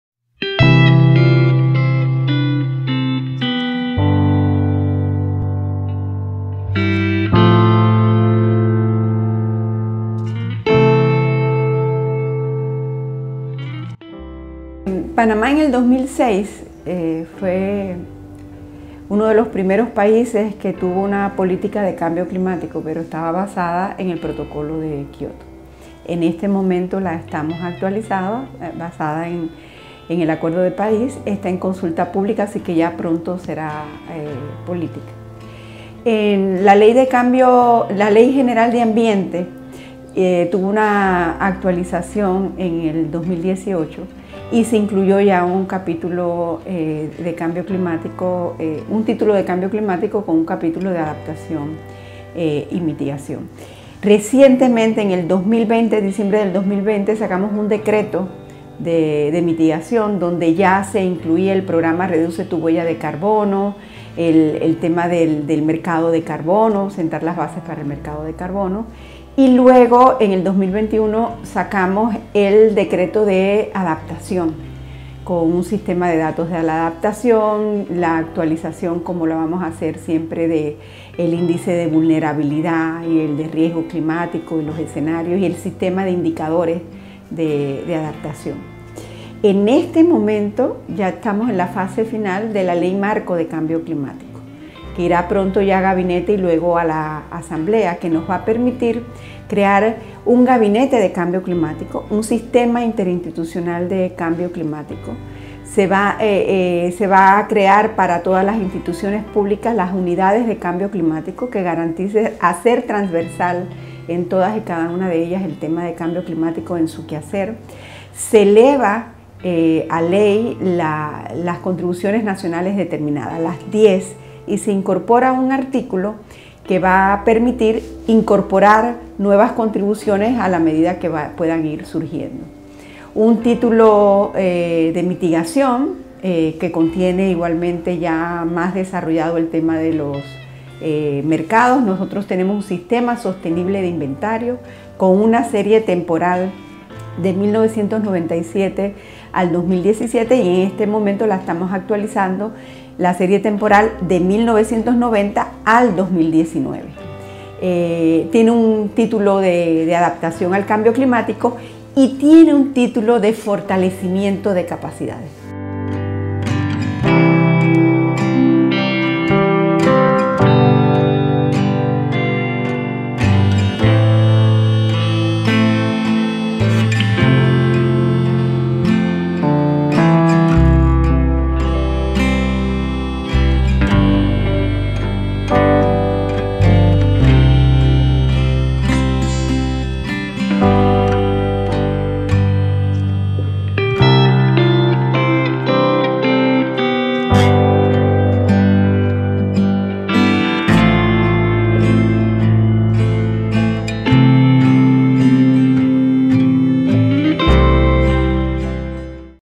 Ley Marco de Cambio Climático de Panamá
Entrevista con Ligia Castro Directora de cambio Climático del Ministerio del Medio Ambiente de Panamá.